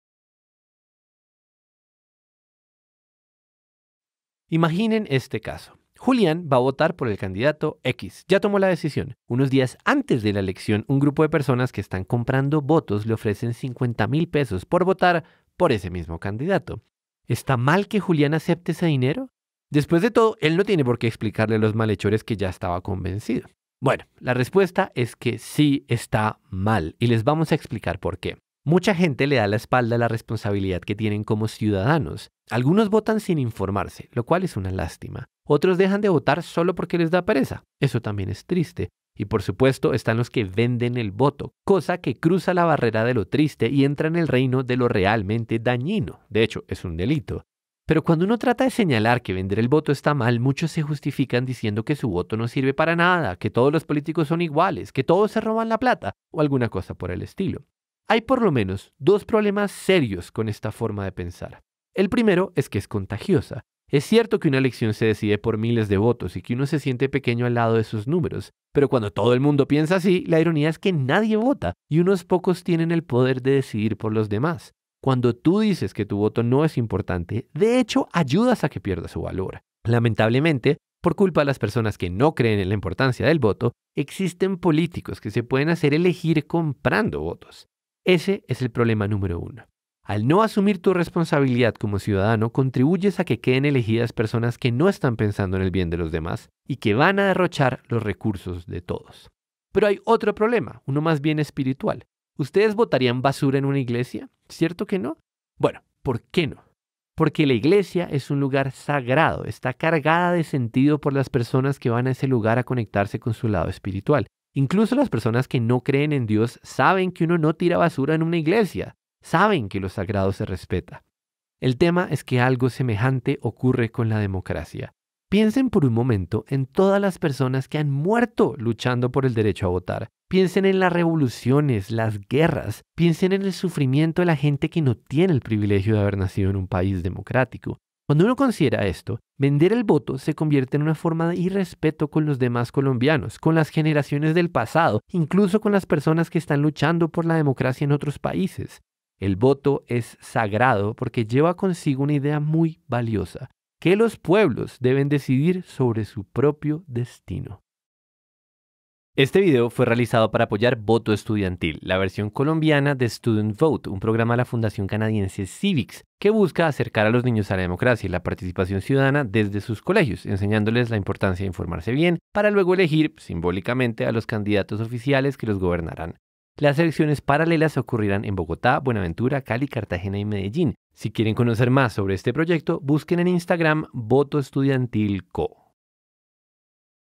¿En cuánto vender el voto?
En Canadá se inventaron un programa para educar a los niños en temas de participación ciudadana, se llama Student Vote. La idea es simular una elección con miles de estudiantes de muchos colegios, pero antes de votar, los estudiantes deben aprender sobre conceptos democráticos.

Este programa está siendo replicado en Colombia y tuvimos la oportunidad de producir varios videos educativos, videos que estamos publicando en nuestro canal porque quedaron muy bonitos... y porque como sociedad todavía nos falta interiorizar estas ideas.